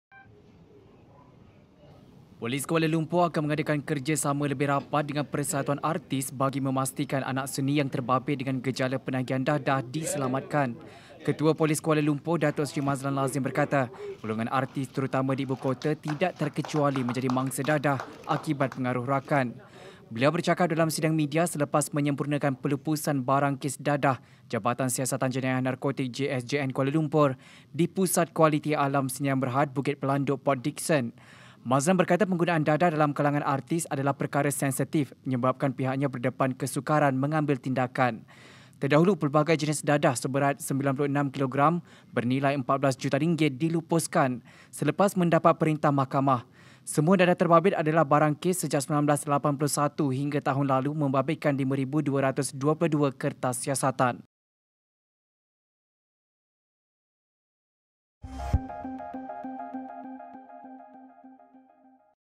Persatuan artis, polis kerja sama selamatkan artis terbabit dadah
Polis akan mengadakan kerjasama lebih rapat dengan persatuan artis bagi memastikan anak seni yang terbabit dengan gejala penagihan dadah diselamatkan.